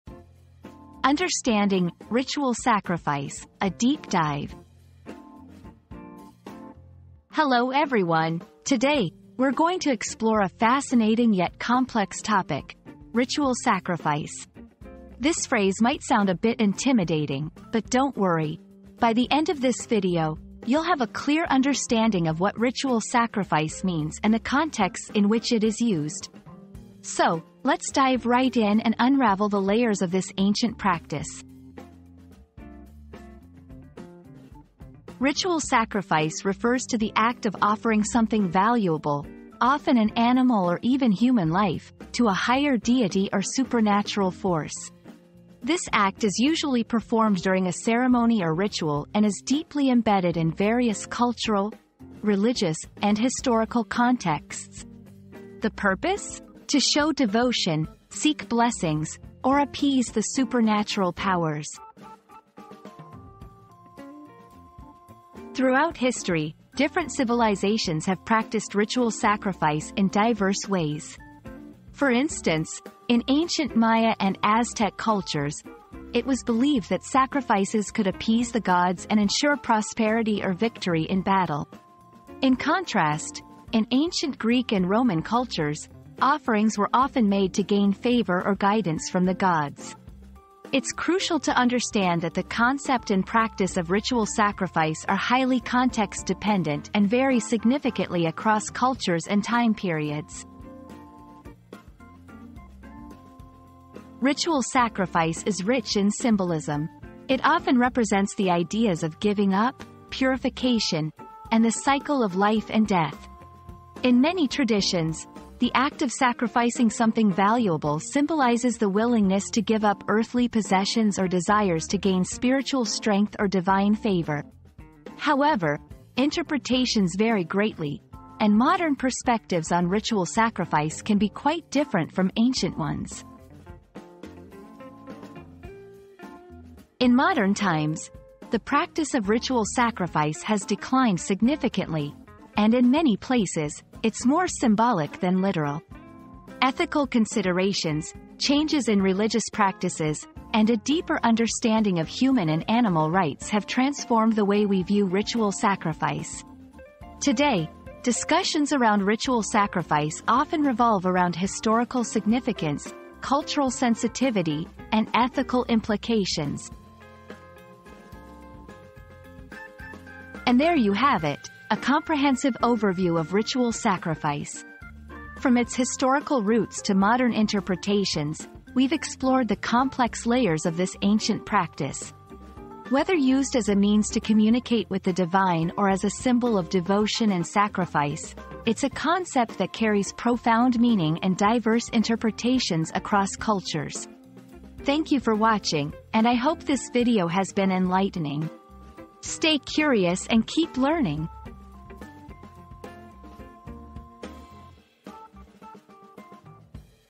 Understanding "Ritual Sacrifice": A Deep Dive
Unraveling the Mysteries: A Deep Dive into Ritual Sacrifice • Embark on a captivating journey as we delve into the enigmatic realm of ritual sacrifice, exploring its origins, meanings, and controversial practices throughout history. Gain a deeper understanding of this ancient ritual and unravel the secrets that lie within.

00:00 • Introduction - Understanding "Ritual Sacrifice": A Deep Dive
00:34 • What is Ritual Sacrifice?
01:05 • Historical and Cultural Context
01:43 • Symbolism and Interpretation
02:16 • Modern Perspectives and Ethical Considerations